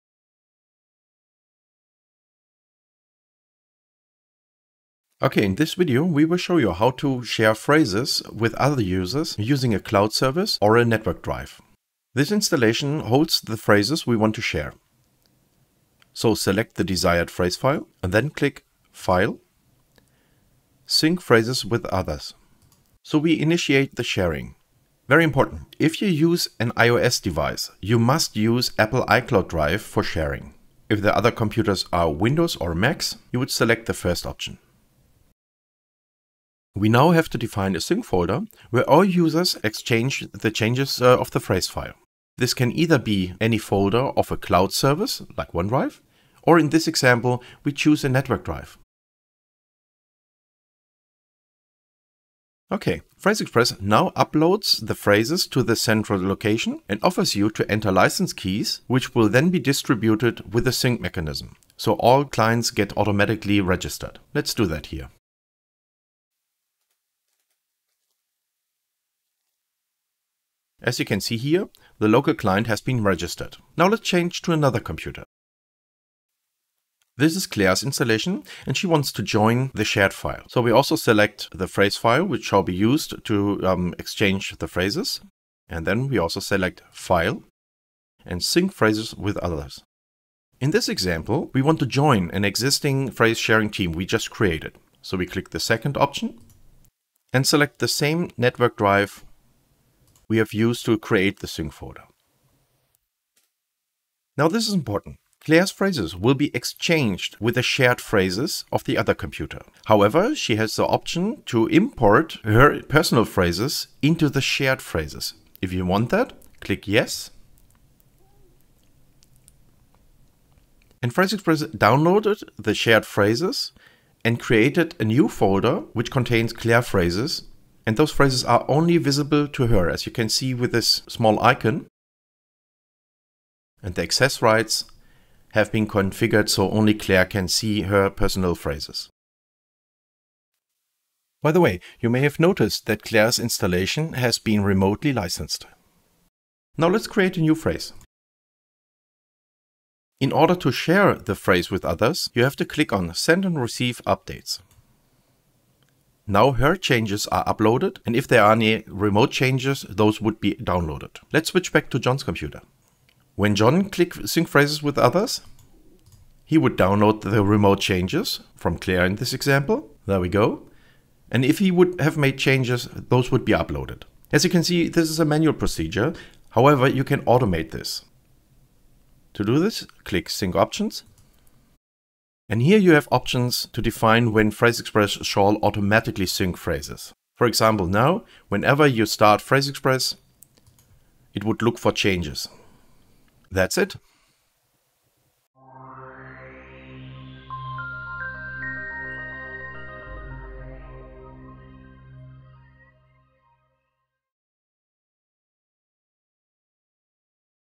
How to setup phrases sharing with text expander PhraseExpress
This tutorial shows you step-by-step how to setup text expander PhraseExpress to share phrases with other users by using any cloud file sync service or a network drive.